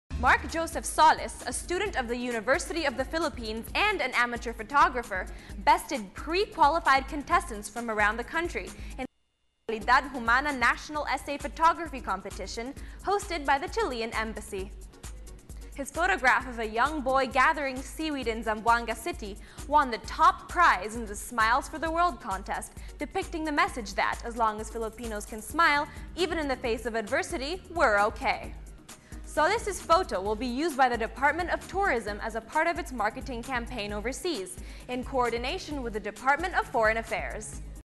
N@1 Junior: Mark Joseph Solis wins in the first Calidad Humana Nat'l Photography Essay Competition
N@1 Junior: Mark Joseph Solis bested 30 pre-qualified contestants in the first Calidad Humana Nat'l Photography Essay Competition (Reported by Chantal Marauta) [September 22, 2013]